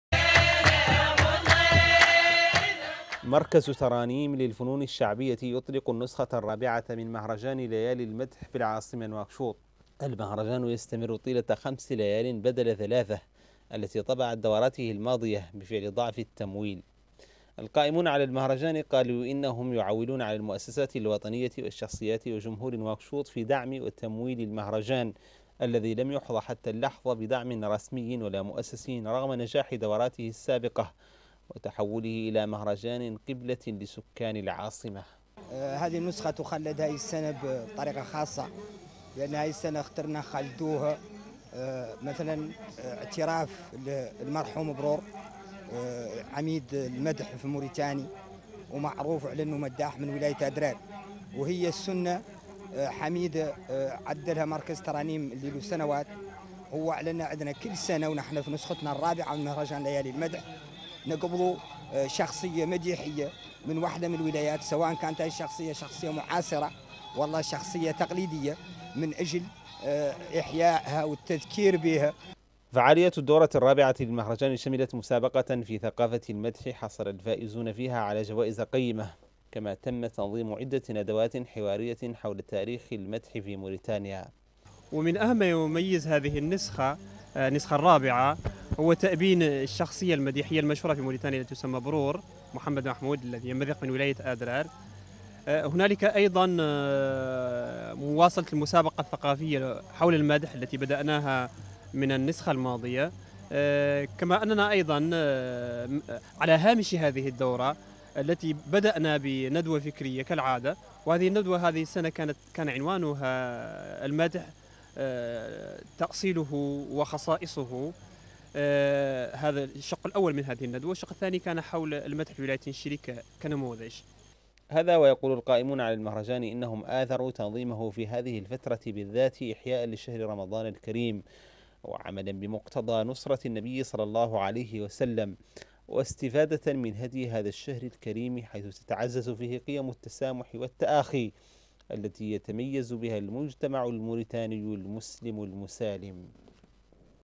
الدورة الرابعة من ليالي المدح في نواكشوط تقرير محمد يسلم ولد خالد.
تتواصل في العاصمة نواكشوط أعمال الدورة الرابعة من النسخة السنوية لليالي المدح، المنظمة من طرف مركز ترانيم للفنون الشعبية.
وتضمنت نسخة هذا العام تنظيم نقاش حول مدلول وخصائص المدح، إضافة إلى تنظيم سهرات مديحية ينعشها عدد من المداحين.
تقرير: محمد يسلم ولد خالد.